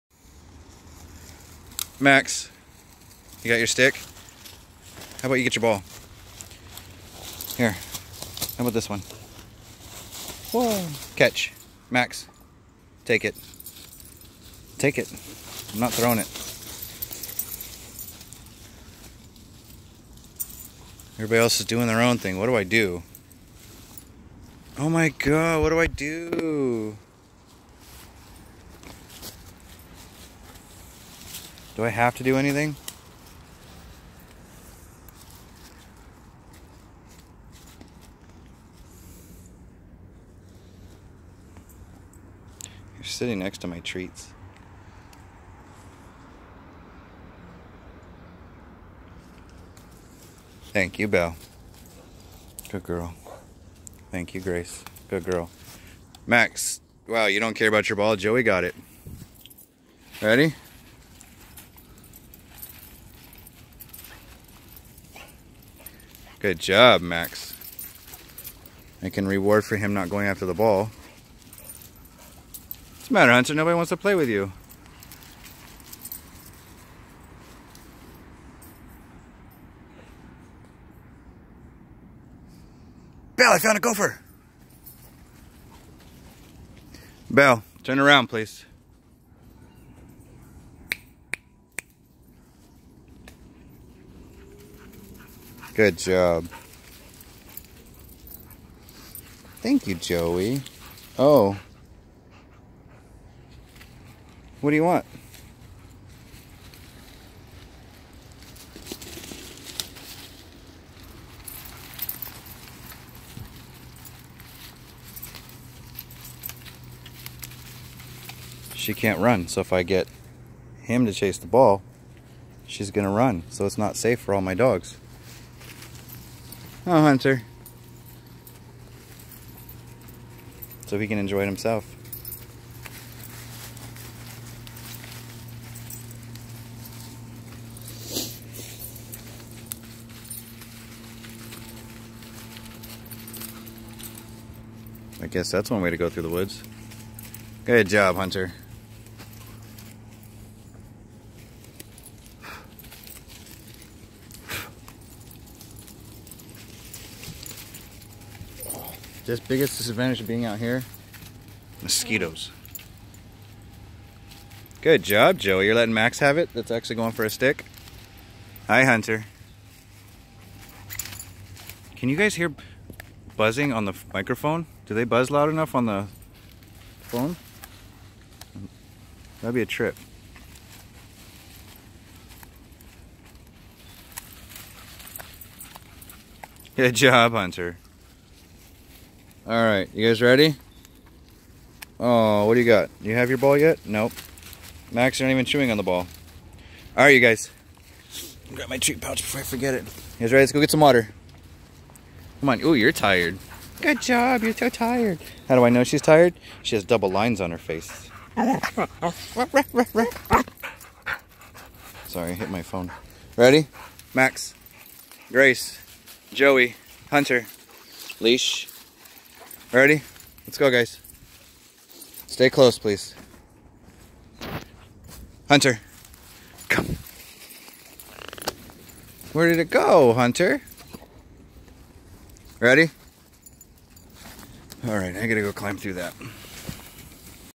Raw and Uncut...Puppy Play
Not really puppies, but there's play.
There's dogs doing nothing.
More importantly?
Me doing nothing.